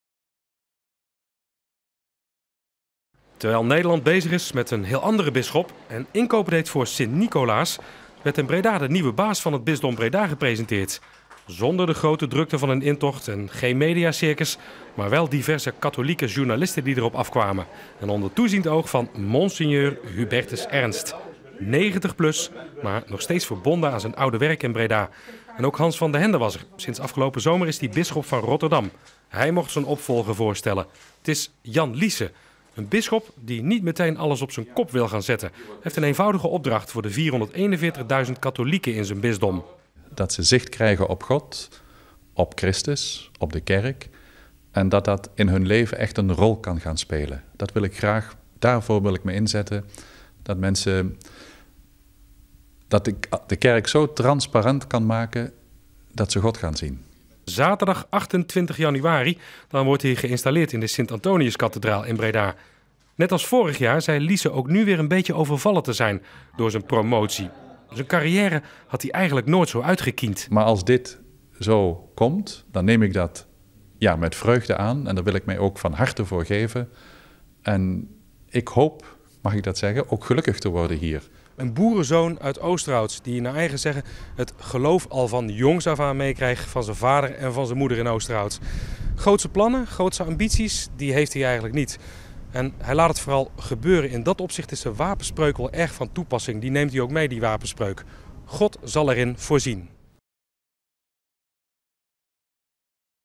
Jan Liesen (51) bisschop van Breda
- De paus heeft een nieuwe bisschop van Breda benoemd. Het is Jan Liesen. Hij is nu nog hulpbisschop in Den Bosch. De bisschop is geboren in Oosterhout en is momenteel 51 jaar oud. Hij volgt Hans van den Hende op. Die is afgelopen zomer naar het bisdom Rotterdam vertrokken.